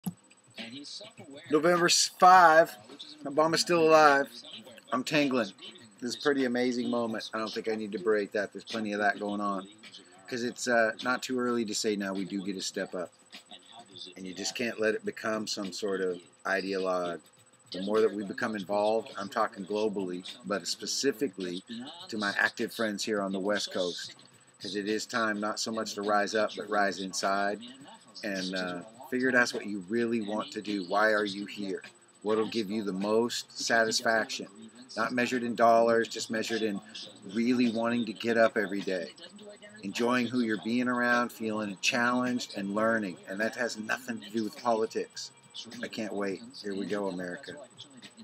Re: Response to Presidential Election 2008
my first take on it..  we danced here too..  thank you my fellow americans!!  hope and change won over hate and fear..  wow i didn't think you had it in ya and america i've always loved you but today i'm soooo proud!!